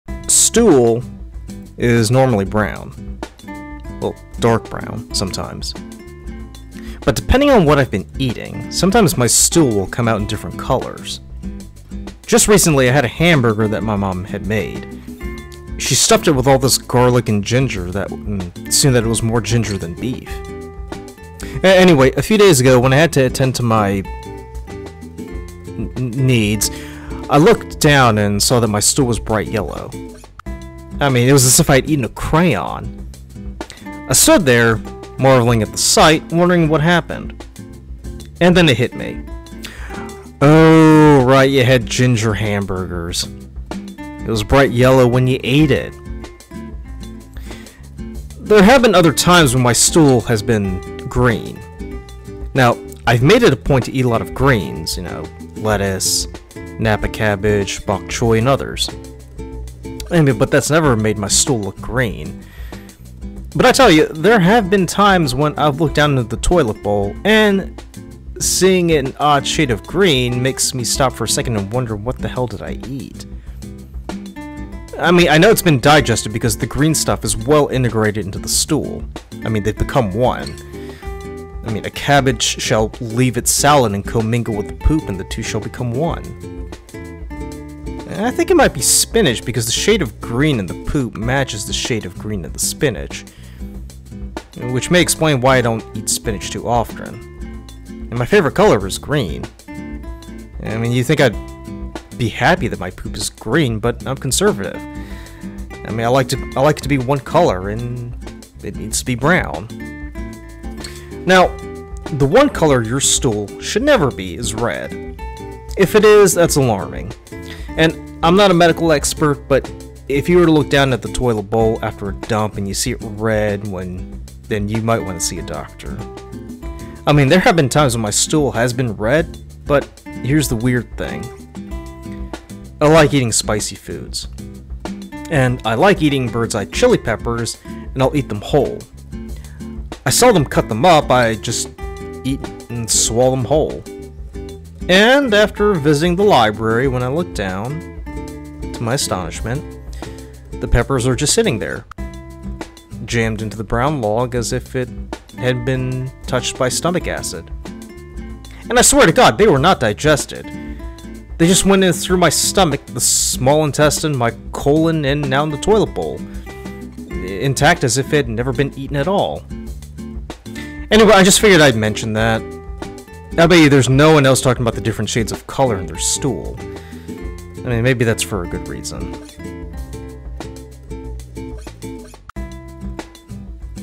Ginger Hamburgers Make Your Poop Yellow
Today I take a look at a very serious matter--does eating ginger actually turn your...droppings yellow? Just the other day I had a hamburger my mother made and it was more ginger than beef. A day later I had to answer nature's call and, to my surprise, saw it was bright yellow. After a few minutes of observing it with intrigue and weirdness, I got the thinking and decided to notate my observations.